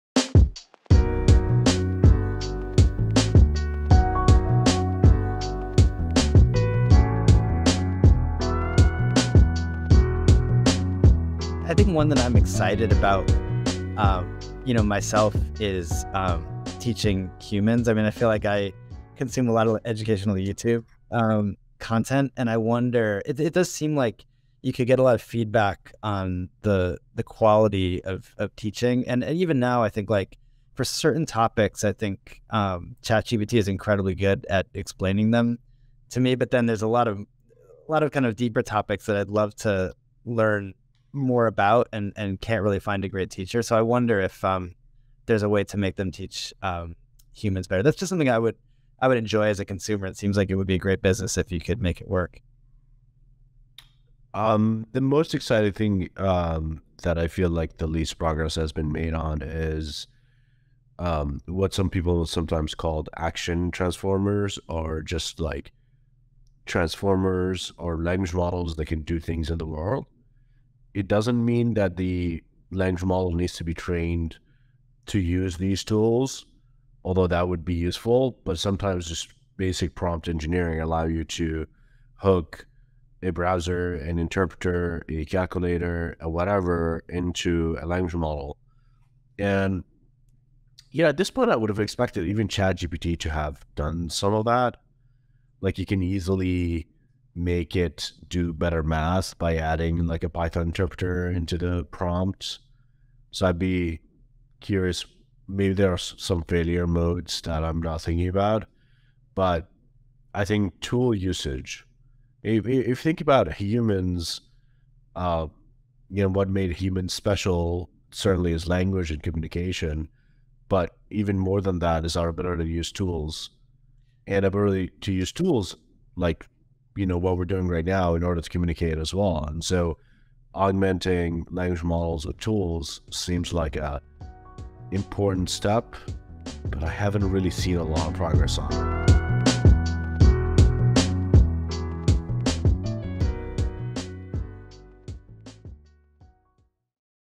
Lukas x Amjad: Exciting Future LLM Use Cases
Join the Weights & Biases x Replit Machine Learning hackathon!
When: Feb 4 - 11, 2023

Watch the full recording of Lukas Biewald and Amjad Masad talk about the Future of ML